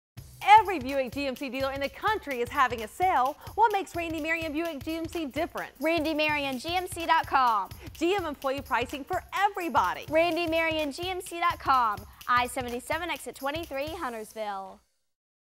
January Buick & GMC Special at Randy Marion
Randy Marion GMC Buick is offering Employee Pricing to everyone in the month of January!